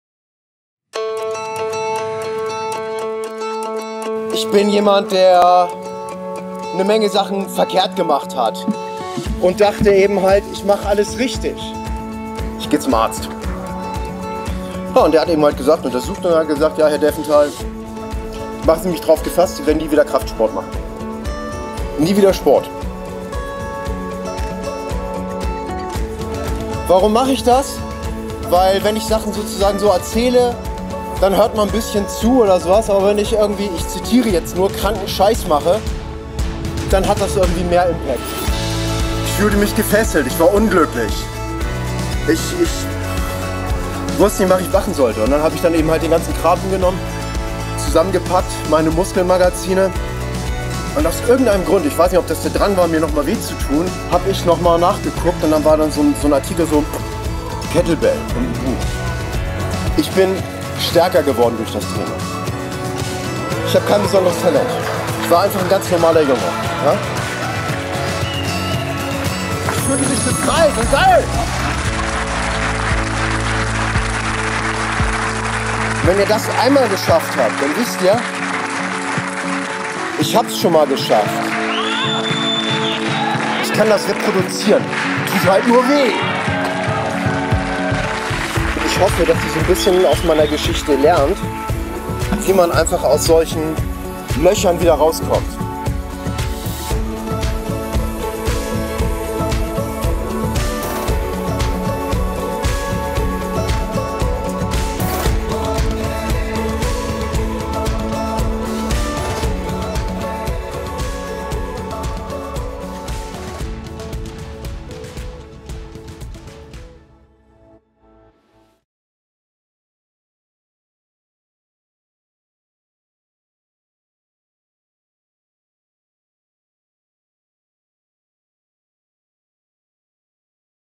Strongman-Frank: Sehen, staunen - reflektieren und handeln (Teaser kurz)
Ich möchte, dass die Leute über sich hinaus wachsen. Kraft und Geist ist kein Widerspruch. Beide profitieren voneinander, wenn die im Einklang sind.
Das ist kein Ergebnis, sondern ein Prozess. Ich helfe Euch mit diesen Weg zu beschreiten. Die Prinzipien sind generell anwendbar ... für Euch muss es kein Kettensprengen sein. Vielleicht ist es für Dich malen oder ein Instrument spielen.
Vielleicht auch alles zusammen? :-)